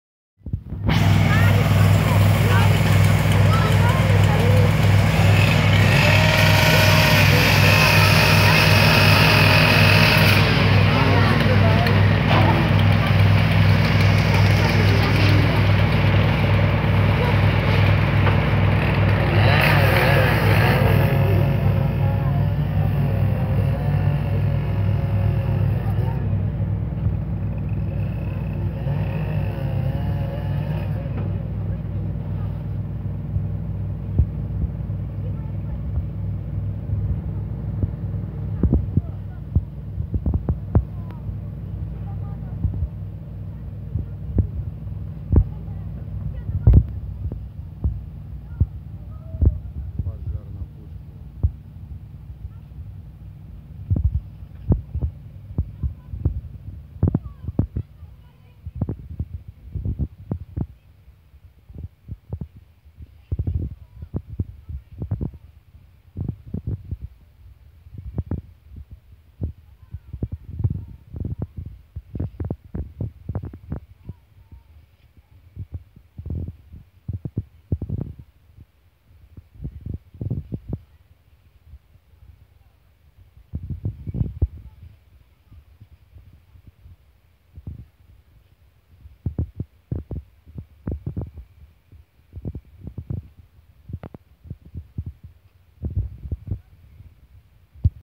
Пожар на улице Пушкина Нижний Новгород
Горят сараи и гаражи